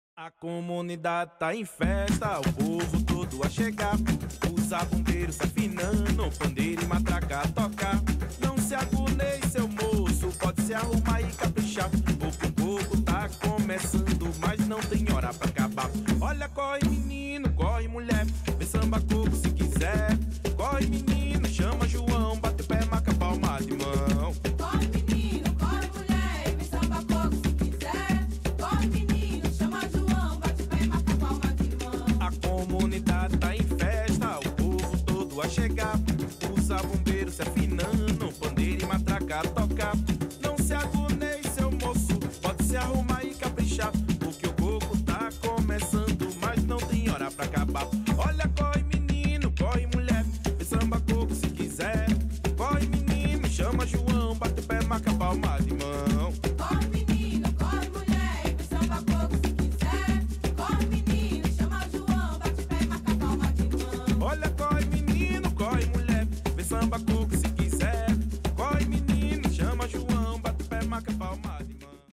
V FESTIVAL DE ARTE E CULTURA NEGRA - FEIRA DAS MULHERES PRETAS & FEIRA LITERÁRIA JEITO XAMBÁ DE SER
V Festival de Arte e Cultura Negra do Cambinda Estrela - " 365 Dias de Novembros Negros contra o apartheid brasileiro"